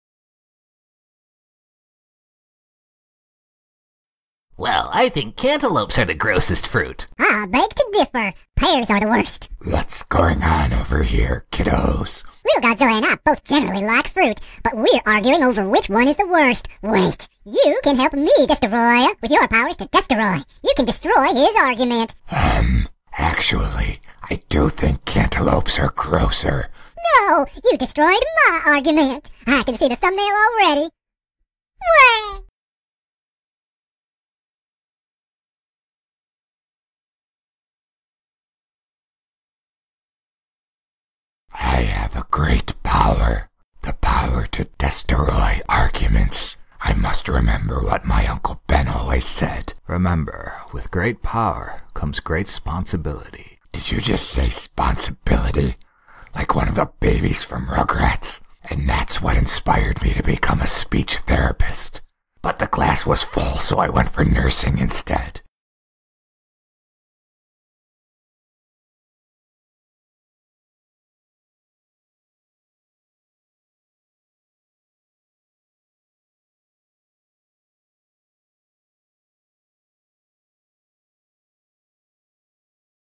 Totally Kaiju Toons vs. Destoroyah's Secret Power!
Watch TKToons DESTROY the destroyers with a literal destroyer, Destoroyah...

God bless,
TKToons